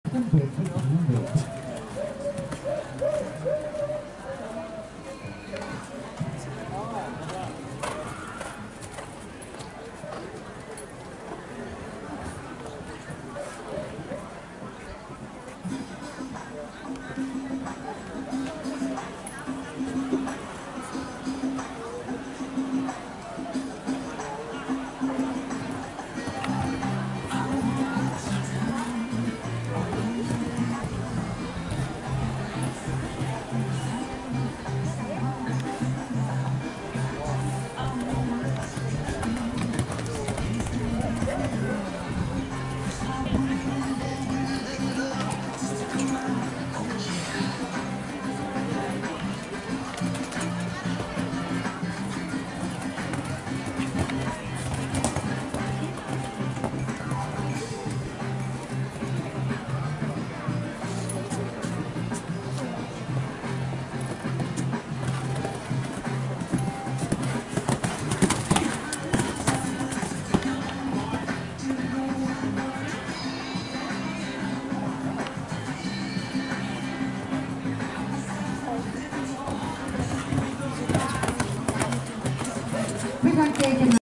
Canturello di luna 4y old by jiqueenturo di luna
canturello di luna clear in the class 4y old horses 31/07/2021 in leopoldsburg